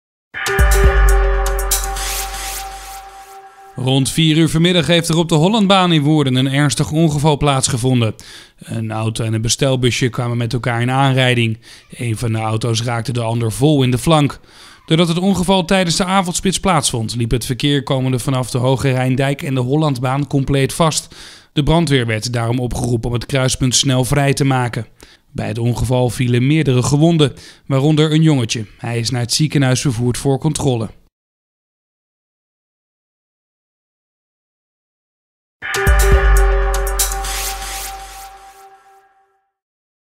Woerden TV | Ongeval op Hollandbaan
Het verkeer in het centrum van Woerden heeft veel overlast gehad van een ongeval op de Hollandbaan. Twee auto's kwamen met elkaar in aanrijding. Hierdoor raakte diverse mensen gewond.